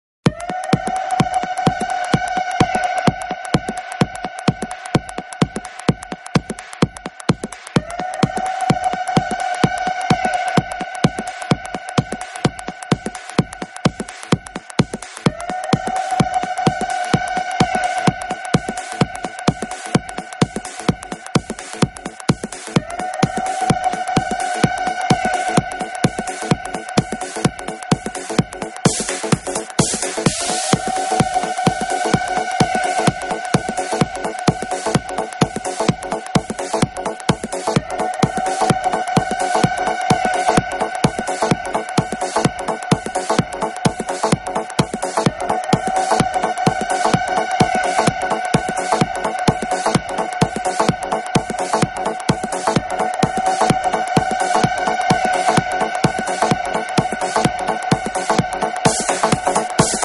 Syncopate @ Velvet Aviano 2010
party di chiusura della stagione house @ Velvet Club

Video realizzato by studiosquare.it